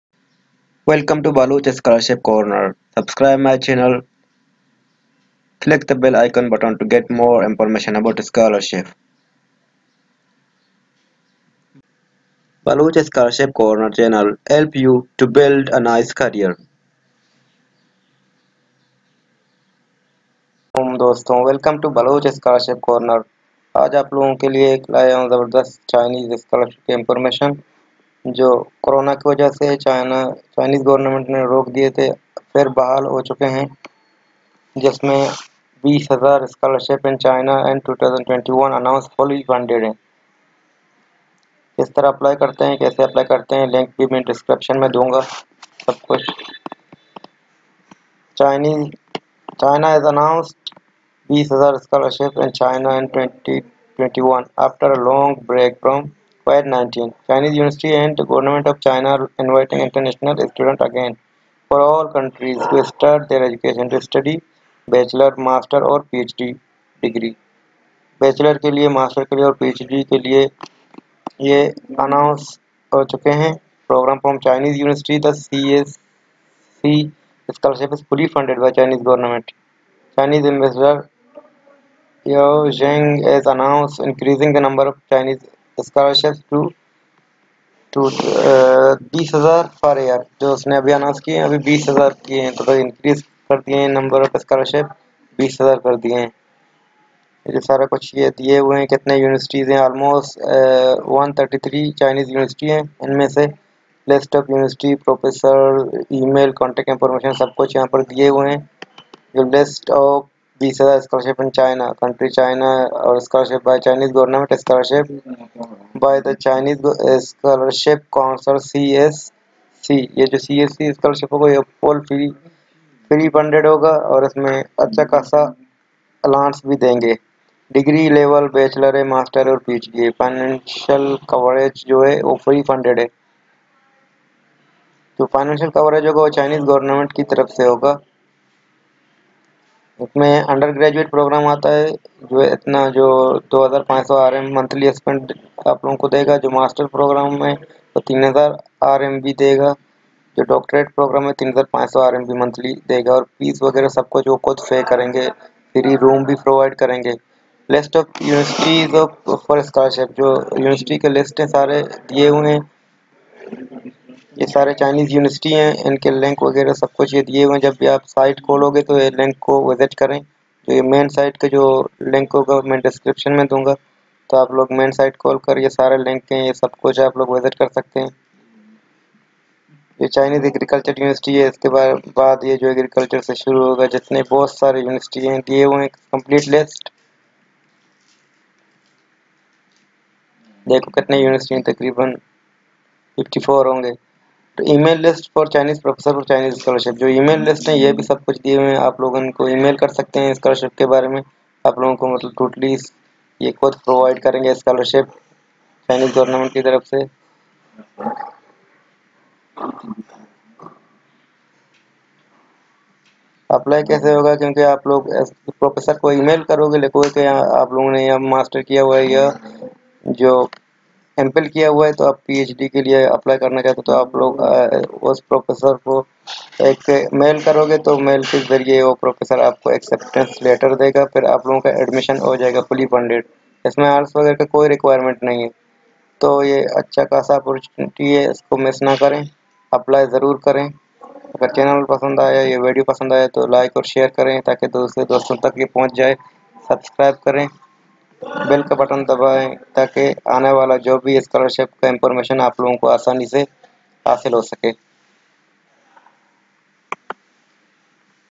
Scholarships in China in 2021 Announced 20000 | Fully Funded
China has announced 20000 Scholarships in China in 2021 after a long break from COVID-19. Chinese Universities and the Government of China are inviting international students again from all countries to Start their Education to Study Bachelors, Masters, or Ph.D. Degree Programs from Chinese Universities. The CSC Scholarship is Fully Funded by the Chinese Government. Chinese Ambassador, Yao Jing, has announced increasing the number of Chinese scholarships to 20,000 per year.
This number is the highest for any country in the world. The Chinese dignitary made this announcement. “It is the people who matter in the relationship,” said the ambassador. You will be eligible to Study at any Chinese university at any academic program. The programs in which you are eligible to Study Bachelors, Masters, or a Ph.D. Degree Programs. Currently, almost 133 Chinese Universities that are accepting Masters and Doctorate students under CSC & many other Scholarships.

China is an increasingly popular destination for students from around the world, with the number of international students in China doubling in the past 10 years. The Chinese Government is heavily investing in international students. Chinese universities have a growing reputation. Find the List of Universities that are offering 20,000 Scholarships in China in 2021.
List of 20000 Scholarships in China
Country: China
Scholarship by: Chinese Government Scholarships by the China Scholarship Council (CSC Scholarship)
Degree Level: Bachelors, Masters, PhD
Financial Coverage: Fully Funded.